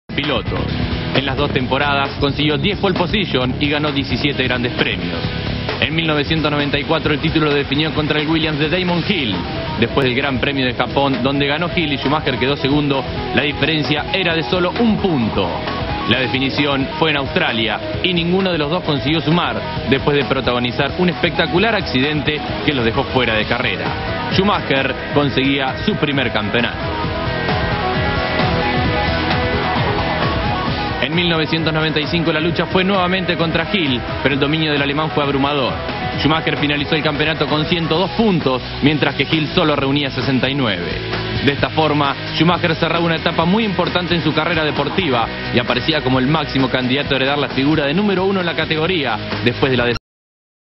M. Schumacher Benetton 94/95
Resumen de las dos temporadas y los primeros dos titulos del alemán en el Benetton-Ford y con el Benetton-Renault